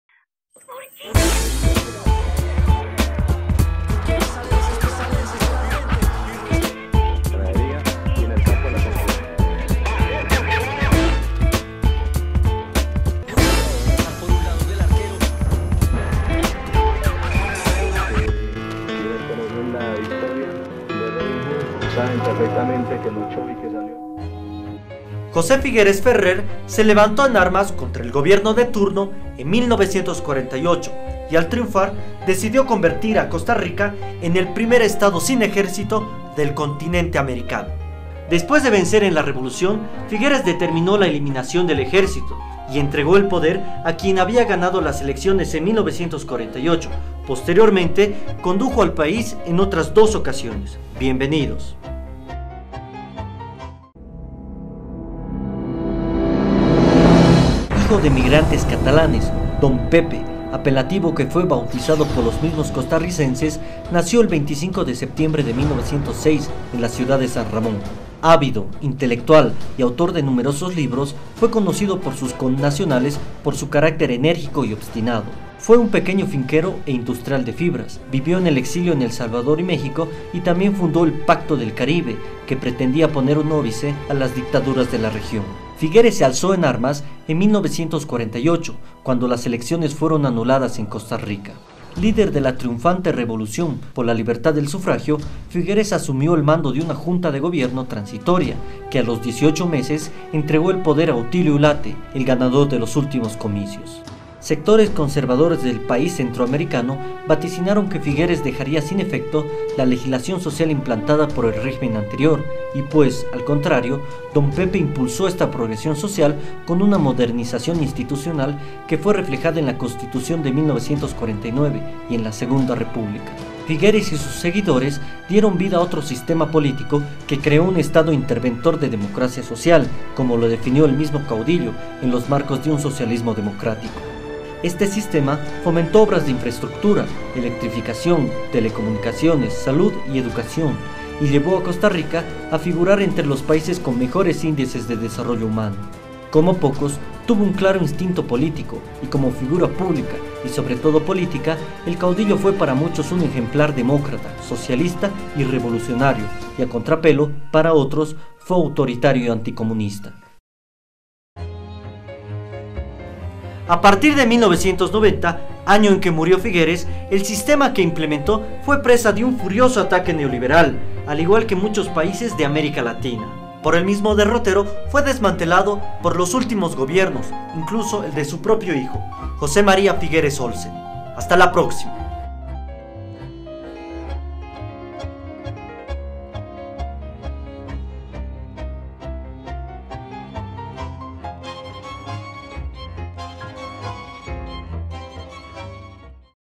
José Figueres Ferrer - Documentales de historia
Costa Rica, en 1948, fue conducida a una revolución. Liderada por 'Don Pepe', eliminó el ejército, implantó un Estado interventor de democracia social...
Producción y dirección: Daniel Cuba Villarroel y Mario Cuba Villarroel